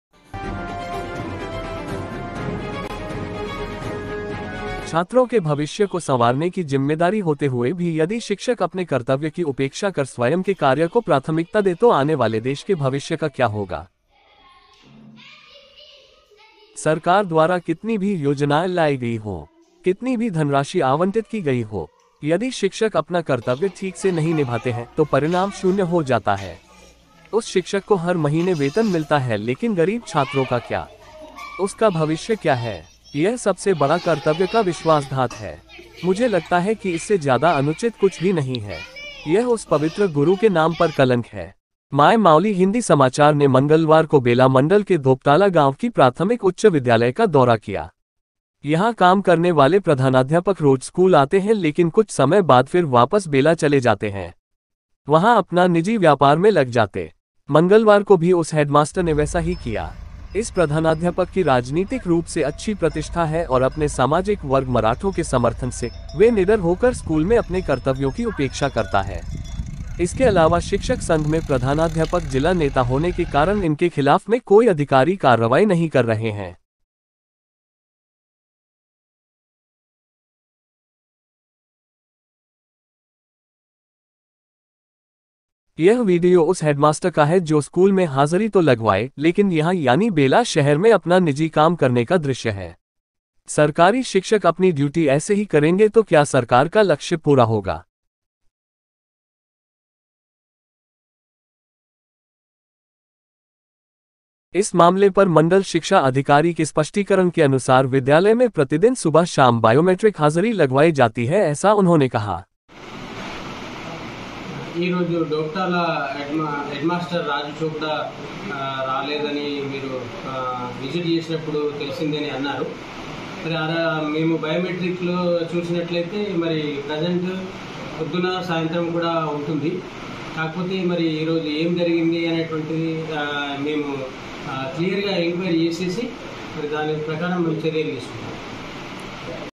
उन गरीब बच्चों को कौन पढ़ाएगा?Who will teach those poor children?
छात्रों के भविष्य को संवारने की जिम्मेदारी होते हुए भी यदि शिक्षक अपने कर्तव्य की उपेक्षा कर स्वयं के कार्य को प्राथमिकता दें तो आने वाले देश के भविष्य का क्या होगा?
 सरकार द्वारा कितनी भी योजनाएं लाई गई हों... कितनी भी धनराशि आवंटित की गई हो... यदि शिक्षक अपना कर्तव्य ठीक से नहीं निभाते हैं, तो परिणाम शून्य हो जाता है।
उस शिक्षक को हर महीने वेतन मिलता है लेकिन गरीब छात्रों का क्या..? उसका भविष्य क्या है?  यह सबसे  बड़ा कर्तव्य का विश्वासघात है...मुझे लगता है कि इससे ज्यादा अनुचित कुछ भी नहीं है...यह उस पवित्र गुरु के नाम पर कलंक है।
 माय माऊली हिन्दी समाचार ने मंगलवार को बेला मंडल के धोपटाला गांव कि प्राथमिक उच्च विद्यालय का दौरा किया।
यहां काम करने वाले प्रधानाध्यापक रोज स्कूल आते हैं लेकिन  कुछ समय बाद  फिर वापस बेला  चले जाते हैं। वहां अपना निजी व्यापार में लग जाते।  मंगलवार को भि उस हेडमास्टर ने वैसा ही किया।
 इस प्रधानाध्यापक की राजनीतिक रूप से अच्छी प्रतिष्ठा है और अपने सामाजिक वर्ग मराठों के समर्थन से, वे निडर होकर स्कूल में अपने कर्तव्यों की उपेक्षा करता है।इसके अलावा शिक्षक संघ में प्रधानाध्यापक जिला नेता होने के कारण इनके  खिलाफ में  कोई अधिकारी  कार्रवाई नहीं कर रहे हैं।
यह वीडियो उस हेडमास्टर का है जो  स्कूल में  हाजरी तो लगवाई, लेकिन यहां यानि बेला शहर में अपना निजी  काम करने का दृश्य है...   सरकारी शिक्षक अपनी ड्यूटी ऐसे ही करेंगे तो क्या सरकार का लक्ष्य पूरा होगा..?